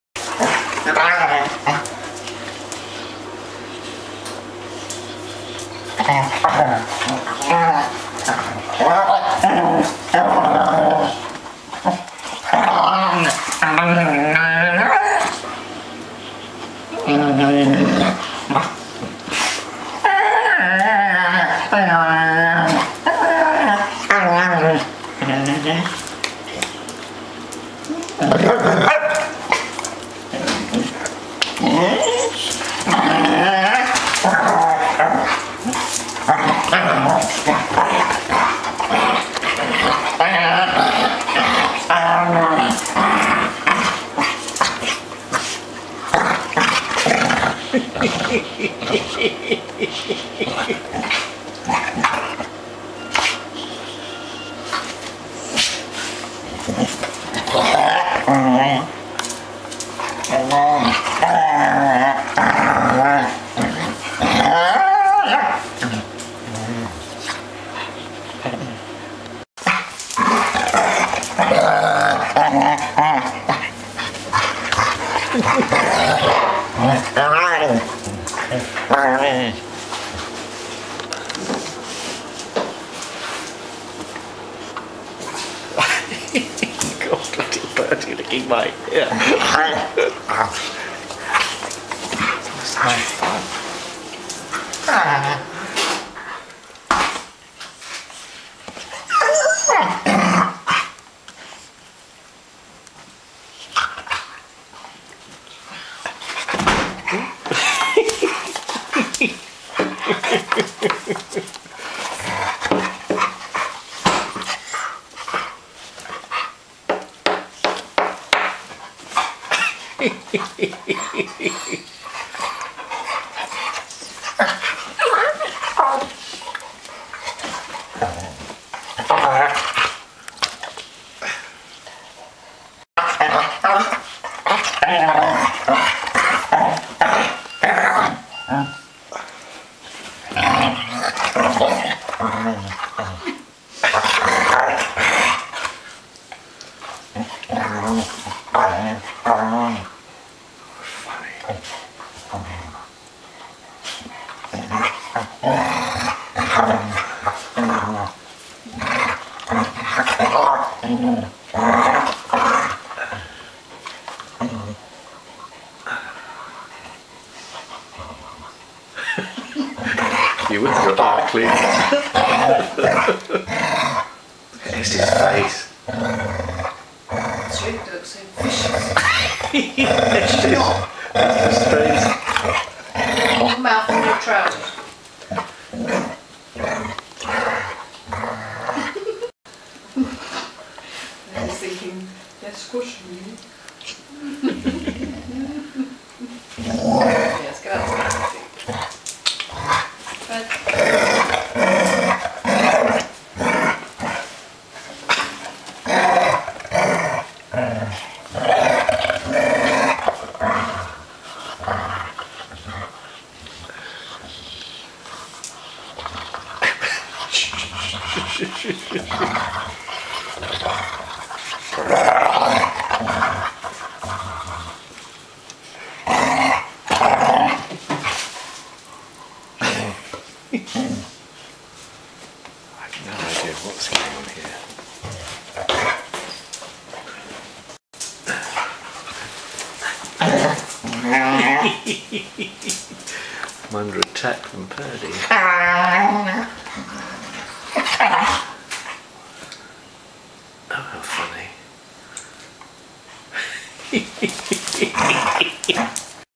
Noisy Dogs
I have no idea what these three were up to but it was obviously fun